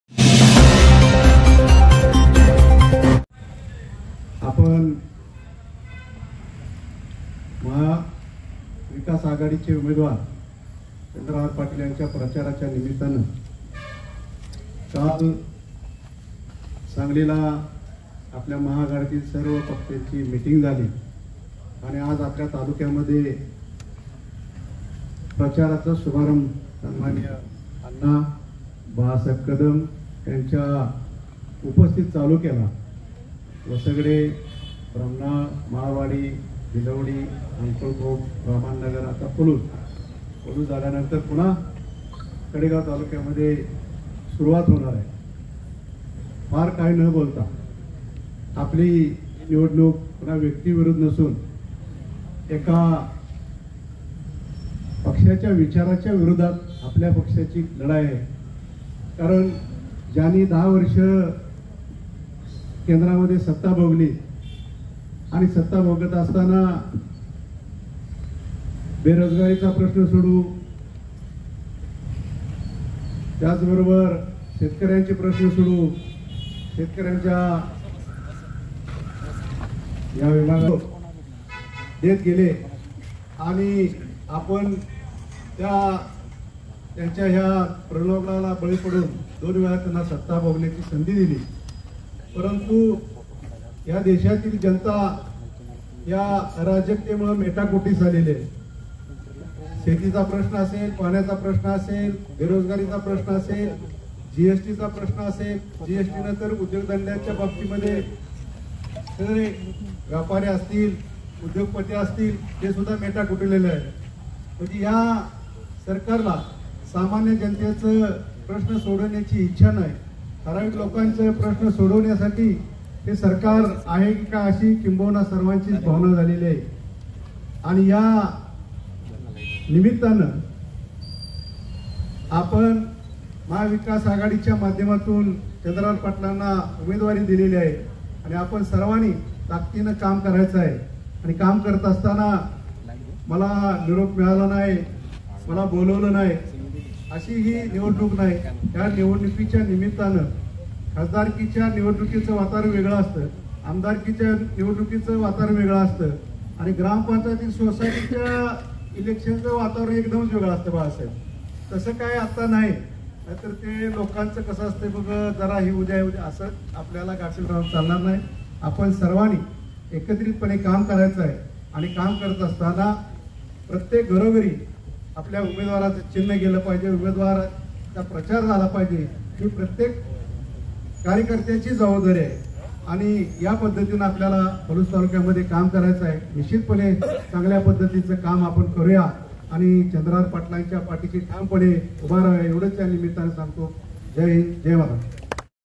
⭕Live | MAHENDRA LAD | पै. चंद्रहार पाटीलांसाठी महाविकास आघाडी पलूस तालुक्यात पहिल्यांदाच एकत्र
महेंद्र लाड | पै. चंद्रहार पाटीलांसाठी महाविकास आघाडी पलूस तालुक्यात पहिल्यांदाच एकत्र